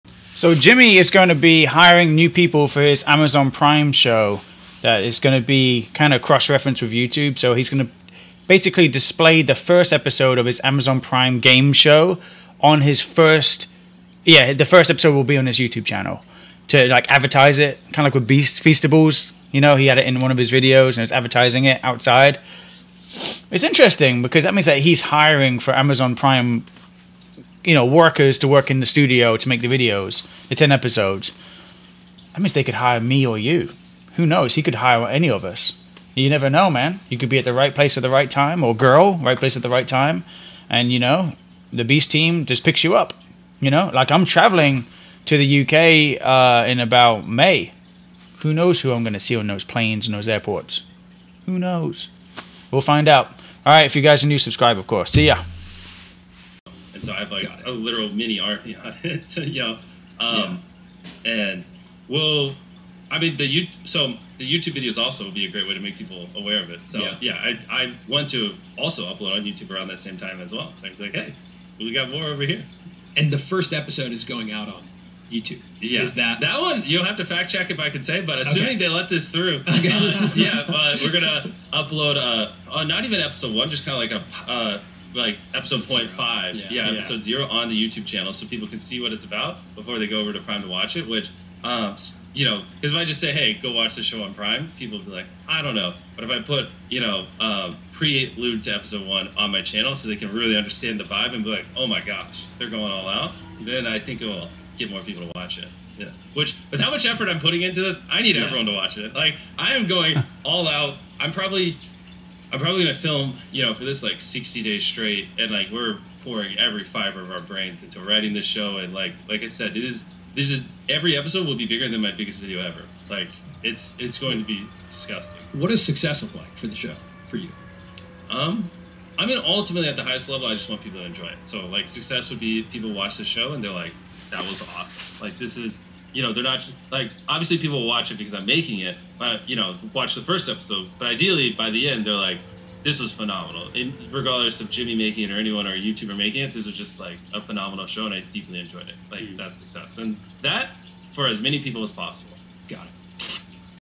MR BEAST IS HIRING US FOR HIS NEW SHOW ON AMAZON PRIME 😗 $$$
Today we briefly talk about mr beast hiring new people, it could be you or me, smash the like and comment, lets see if this short vid goes viral and i get hired for the beast team lol, i would help for free.